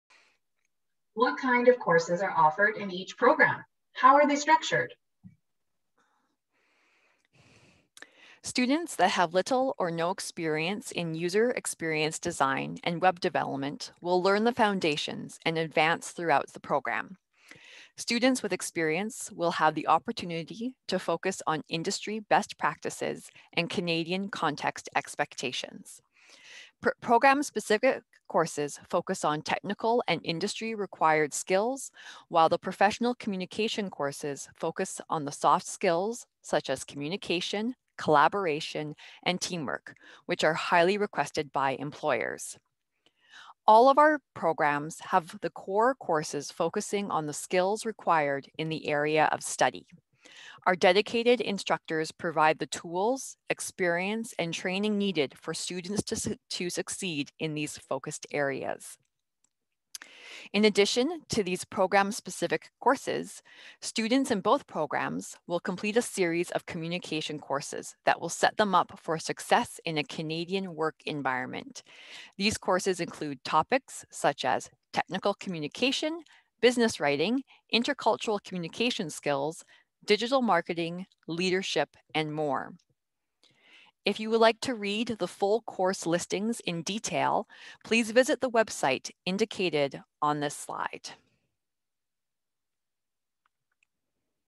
What Kinds of Courses are Offered in the New International Programs?
This video overviews the topics covered in our international professional development certificates, focussing on the two new programs, Digital Experience Design and Full-Stack Web Development. In addition to overviewing program specific courses, this video also emphasizes the professional communication courses offered as a part of each of our programs.

Learn more about our international professional development certificates at MacEwan.ca/SCEInternational